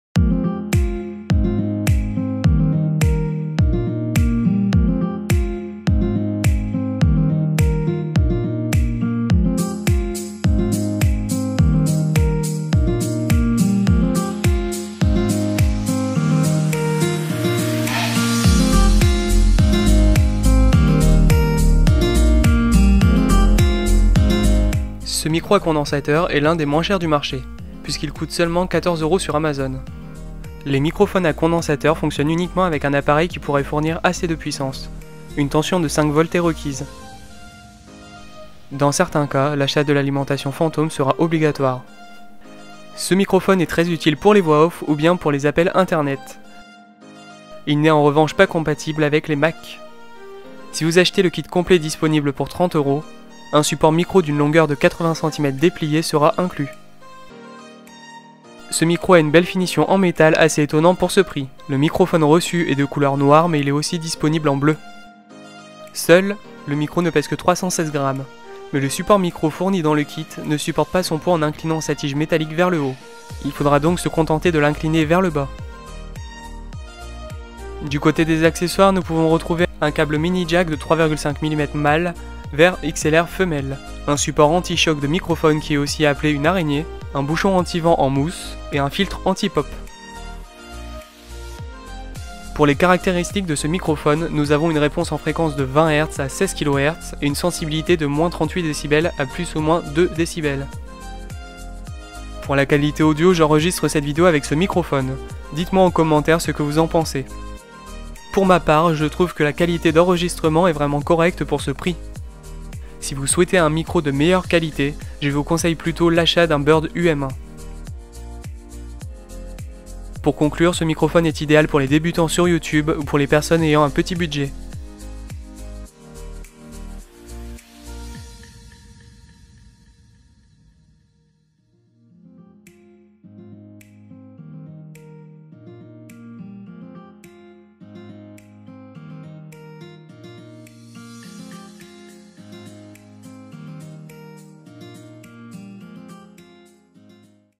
[Test] Un microphone studio a 15 € ?! - Neewer NW-800 (4K)
Aujourd’hui petite vidéo test du microphone Neewer NW-800
Petite nouveauté, la vidéo en 4K et disponible !!

Ce micro est idéal pour les débutants ou pour les personnes ayant un petit budget , mais que vaut-il réellement ? Dans ce test je vous explique les petites choses à savoir avant de passer à l'achat !
En vaut-il vraiment le coup ?

Liens produits :
➡ Neewer NW-800

N'hésitez pas à poser vos questions ou remarque en commentaire !

INFO: les commentaires sont modérés avant leurs mises en ligne